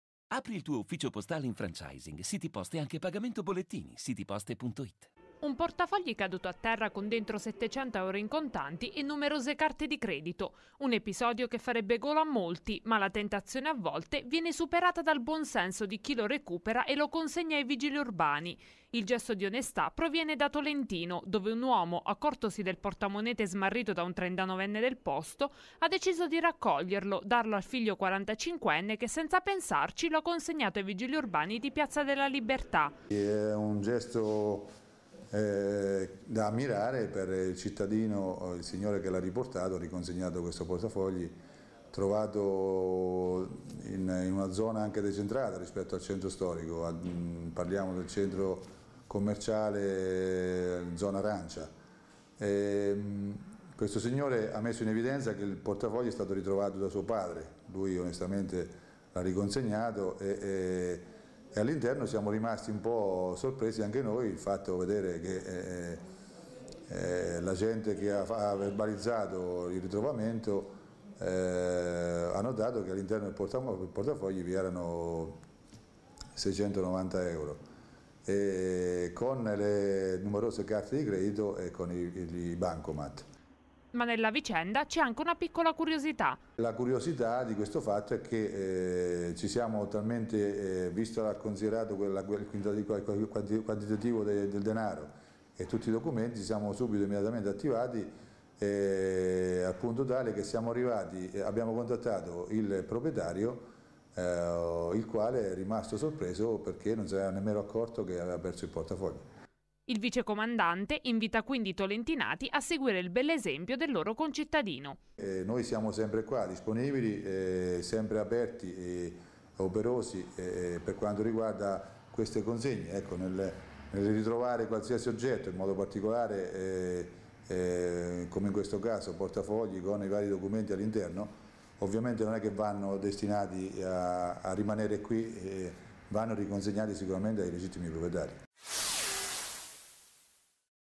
Tolentino - Trova un portafogli con 700 euro, lo consegna ai vigili
Il vicecomandante Rocchetti: "Un bell'esempio"